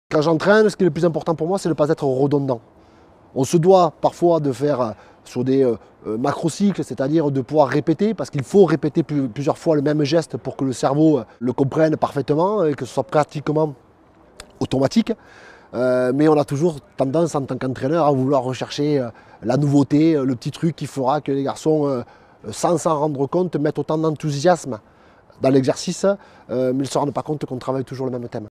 Xavier Garbajosa - Planifier un entraînement
🧠🏉 Xavier Garbajosa, manager général du LOU Rugby explique comment planifier au mieux l'entraînement

ℹ️ Le cours vidéo complet ➡️